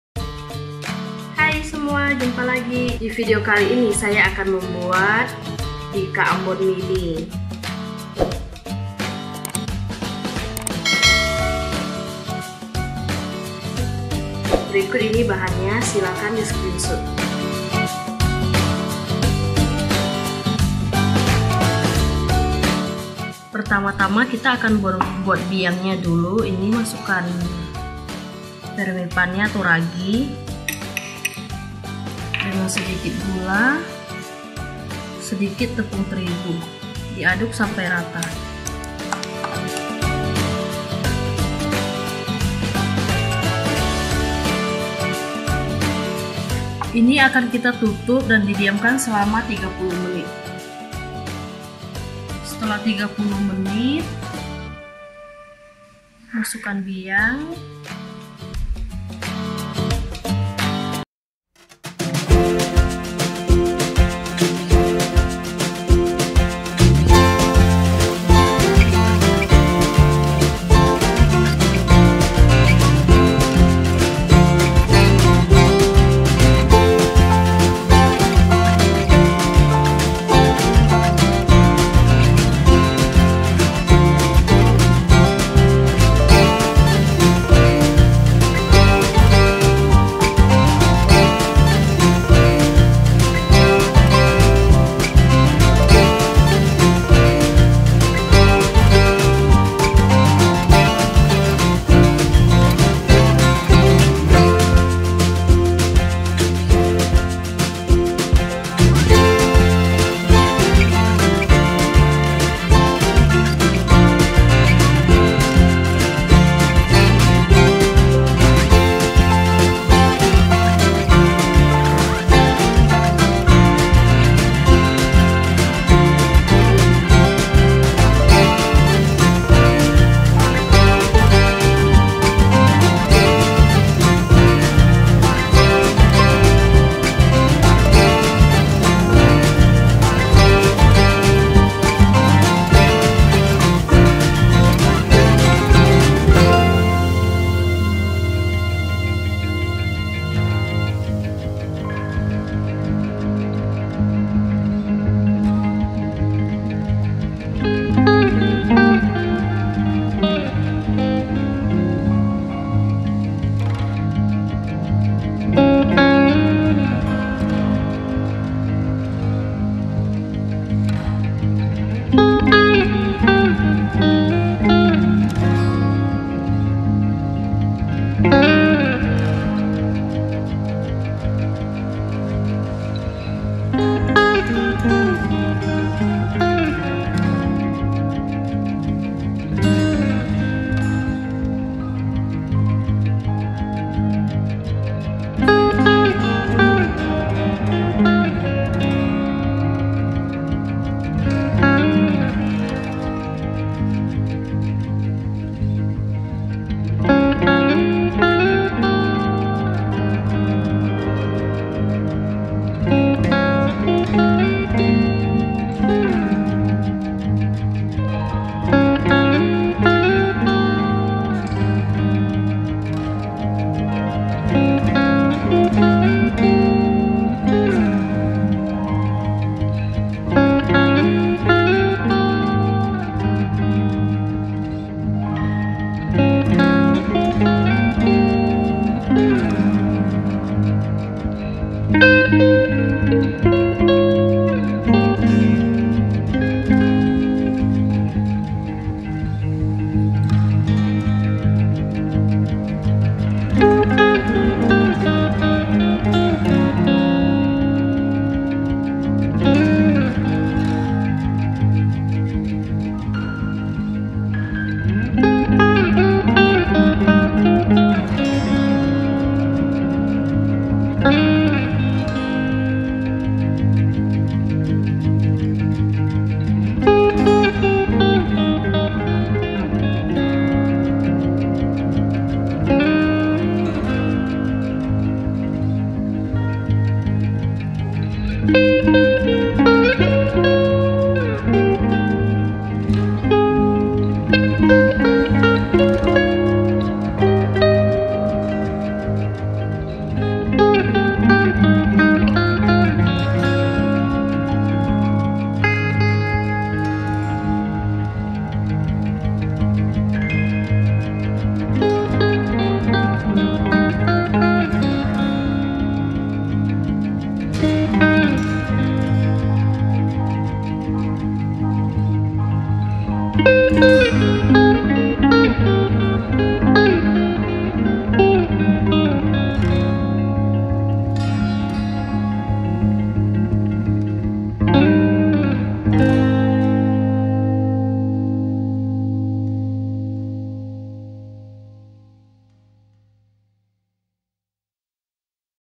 BIKA AMBON MINI EKONOMIS
BAHAN

*TAPIOCA 100 GR
*GARAM 1/2 SENDOK TEH
*MENTEGA 30 GR (CAIRKAN)

*PEWARNA KUNING TUA 4 TETES/JIKA MAU AGAK KUNING BOLEH DI TAMBAH
*SANTAN KENTAL 200 CC
*TELUR UTUH 2 BUTIR
*GULA PASIR 100 GR
*FERMIPAN 6 GR
*AIR HANGAT 60 ML

SIMAK VIDEONYA SAMPAI SELESAI YAH,,,